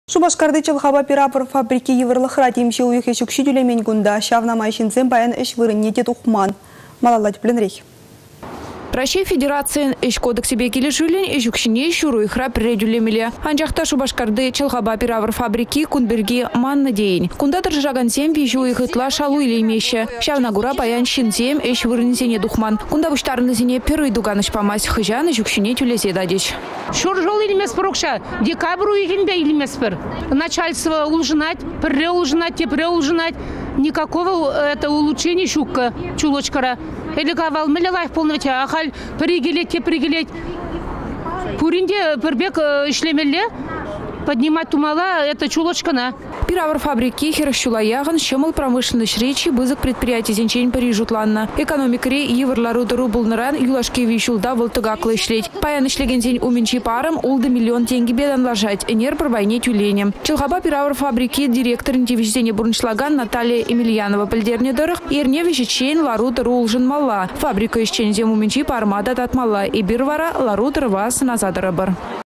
Шупашкарти чăлхапа пир авăр фабрики йывăрлăхра
Шупашкарти чăлхапа пир-авăр фабрики йывăрлăхра. Темиçе уйăх ĕç укçи тÿлемен кунта. Çавна май çынсем паян ĕç вырăнне те тухман.